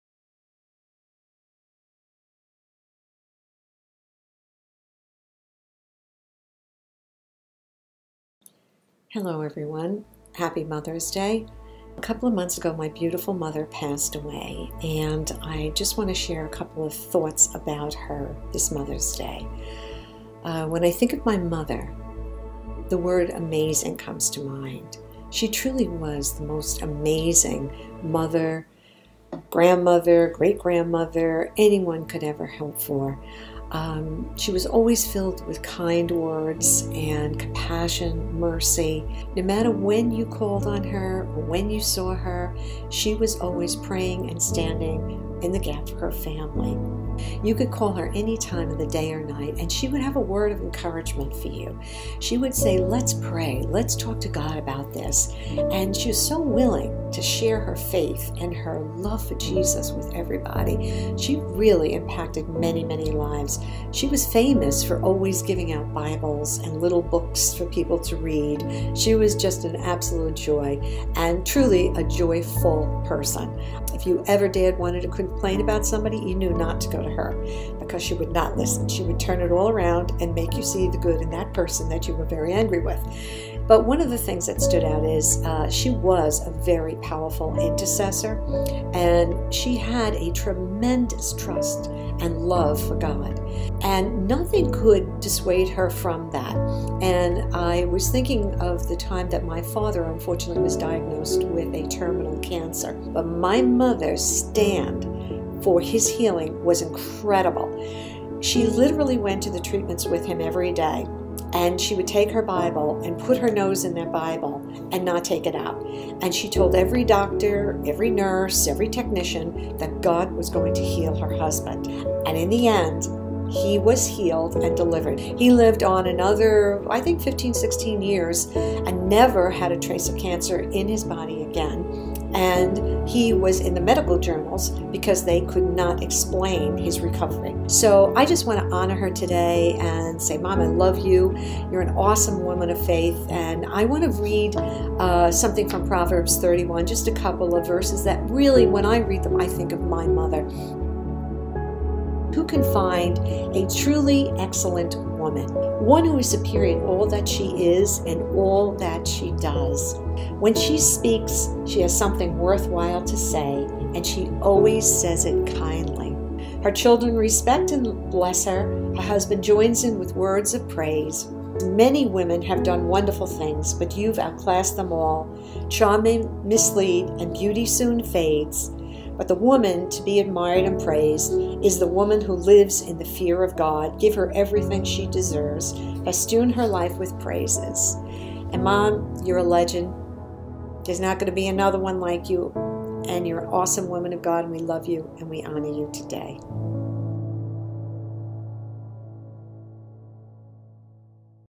Mother's Day 2020: Suzanne's story
We asked Suzanne to share about her mother.